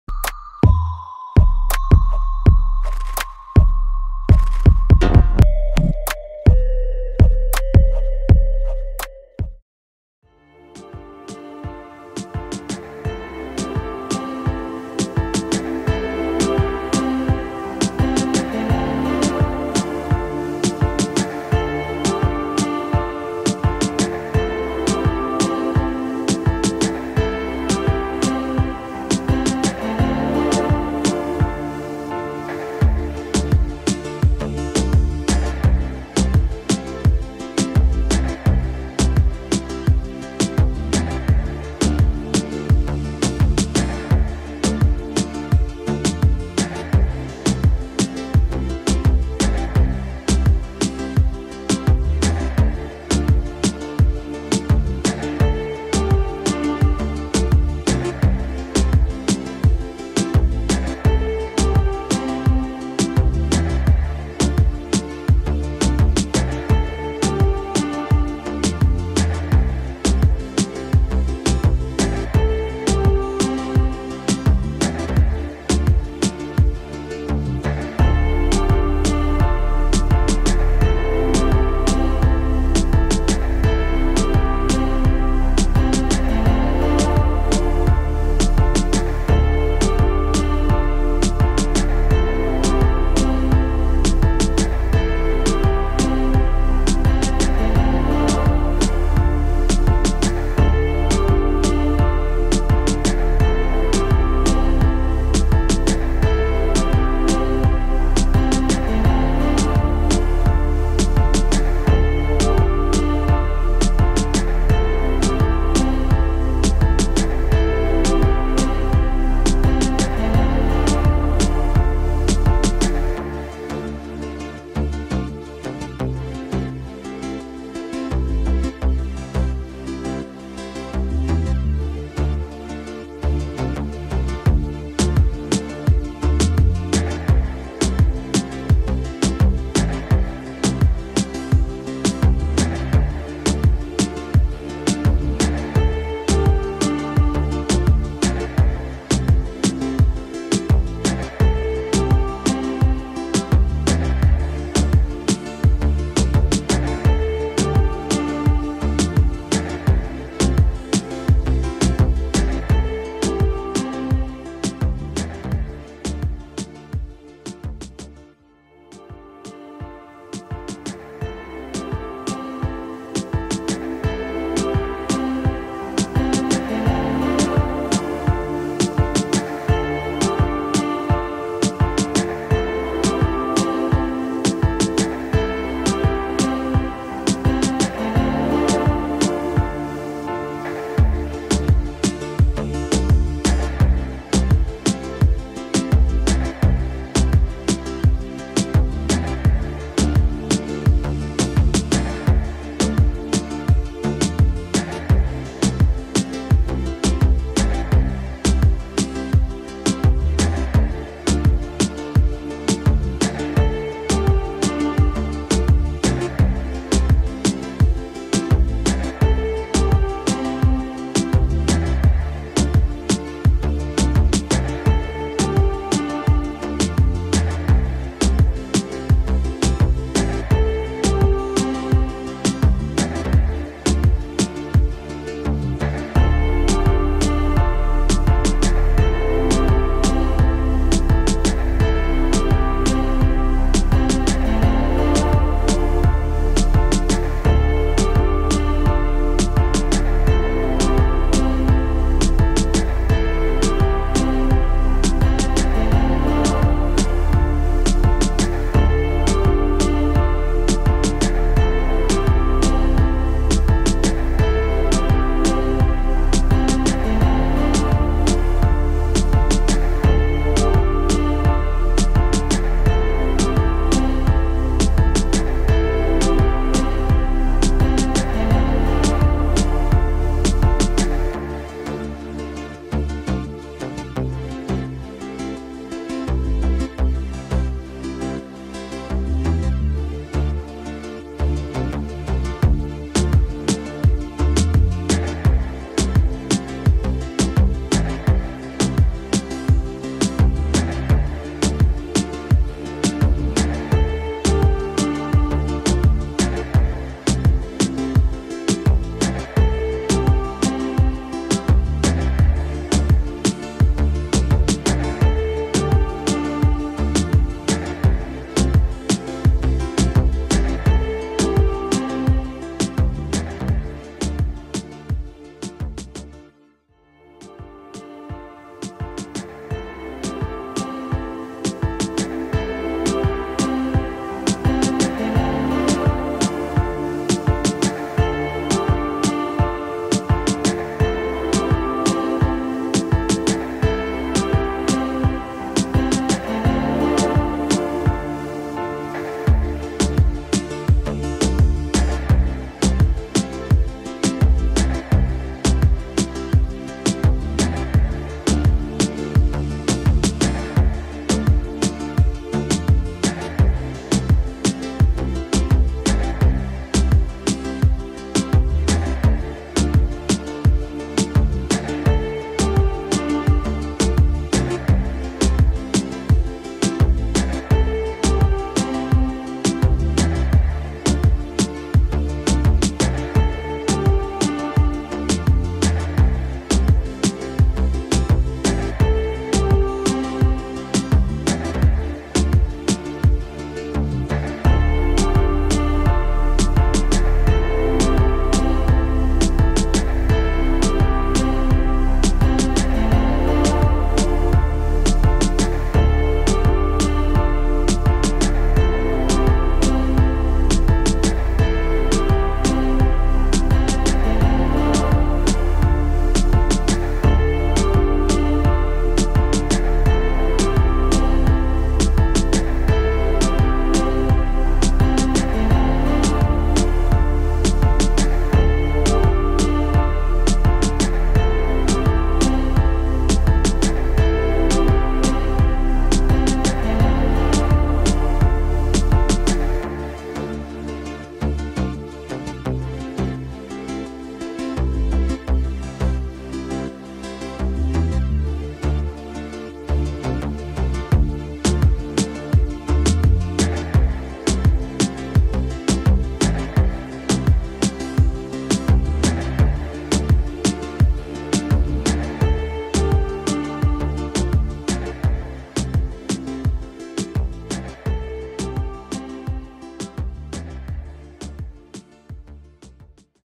Snapseed New Editing Trick | Dual Tone Effect | Snapseed Photo Editing Tutorial
In this video we will show how to make dual tone effect by only using snapseed. This is going to be an unique editing trick.

♻️Paid Editing Available

Hope the video will be helpful.